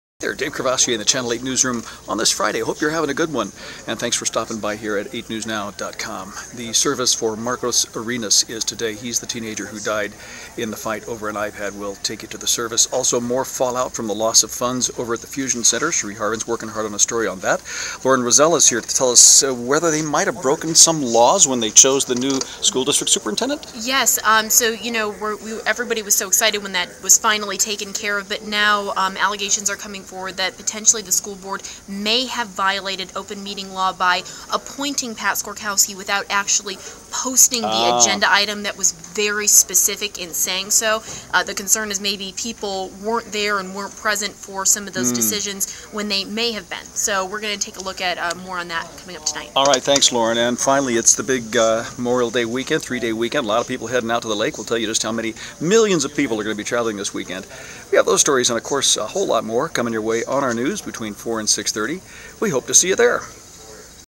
Memorial Day, Marcos Arenas, CCSD, open meeting law, fusion center
What will you be doing this Memorial Day weekend? About 35-million Americans will be traveling, and thousands are headed out to Lake Mead. The fallout continues over the loss of funds to the So. NV Fusion Center. There are now rumblings of a lawsuit being filed over the way the new Clark County School Superintendent was chosen. Some thing the open meeting laws were violated. And today was the funeral for Marcos Arenas, the teen who lost his life after two attackers tried to steal his iPad.